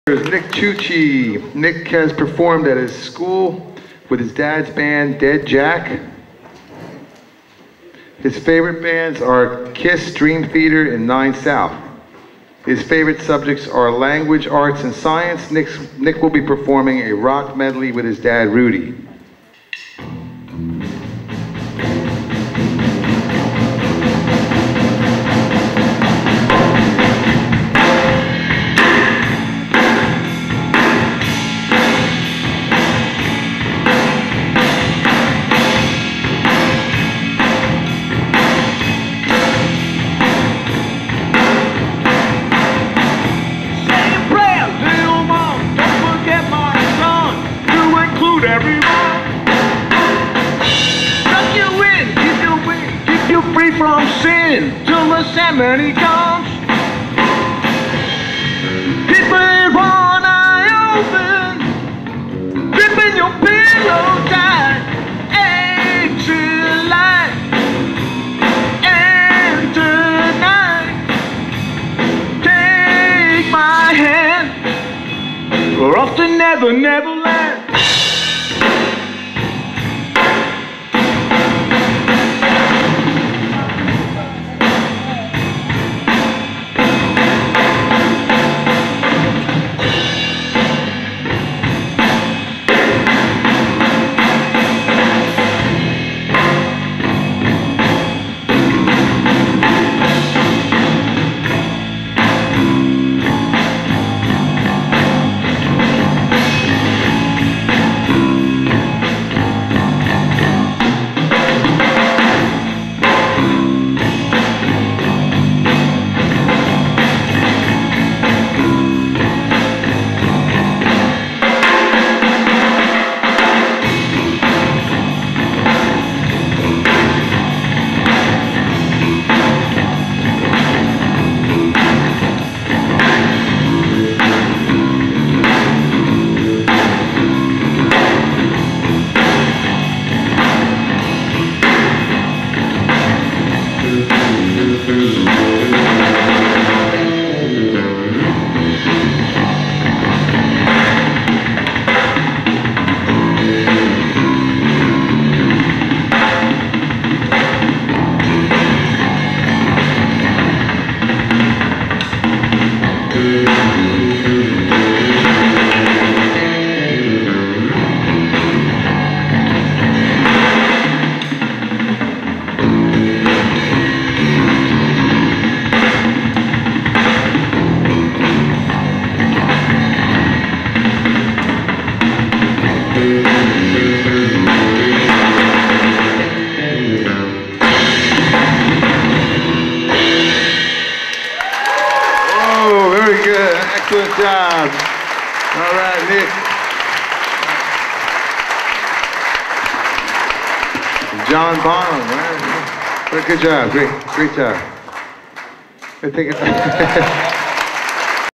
Nick-Tucci-BigBeat showcase-2010.mpg
Nick Tucci performing at the Big Beat showcase 11/13/2010